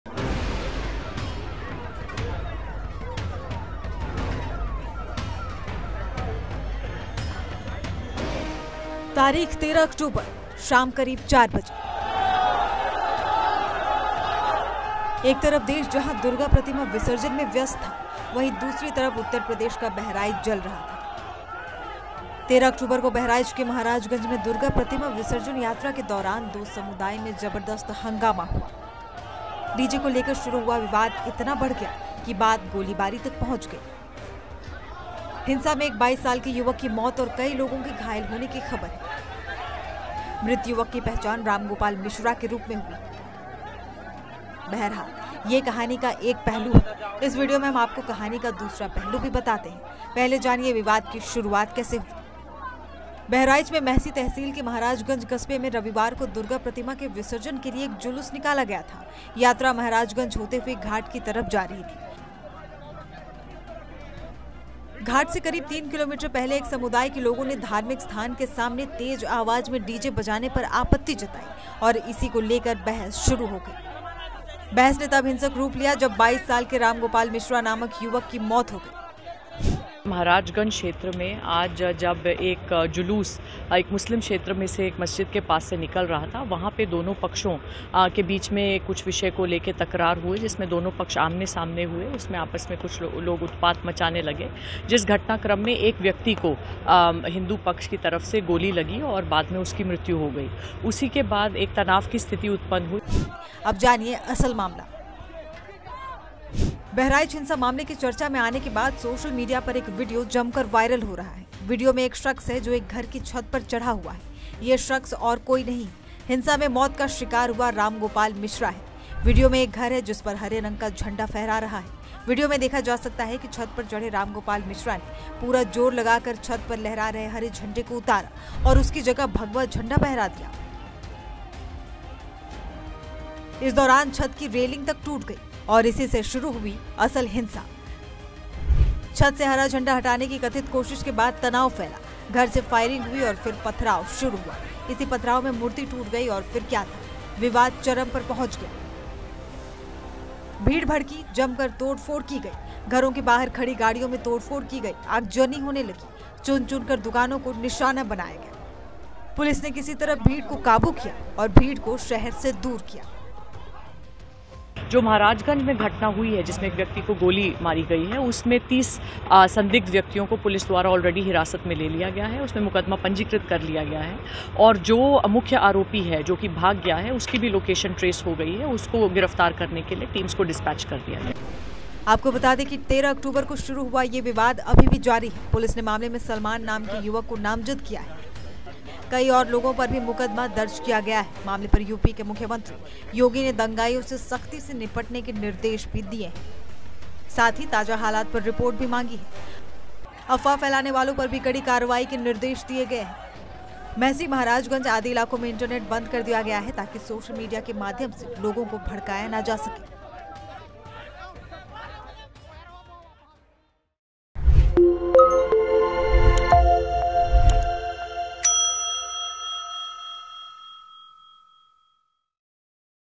Baharich Violence: कैसे हिंसक हो गया बहराइच विवाद | जानिए पूरी कहानी | UP News | CM Yogi
Baharich Violence: बहराइच हिंसा मामले के चर्चा में आने के बाद सोशल मीडिया पर एक वीडियो जमकर वायरल हो रहा है। वीडियो में एक शख्स है जो एक घर की छत पर चढ़ा हुआ है। ये शख्स और कोई नहीं..हिंसा में मौत का शिकार हुआ रामगोपाल मिश्रा है। वीडियो में एक घर है जिसपर हरे रंग का झंडा फहरा रहा है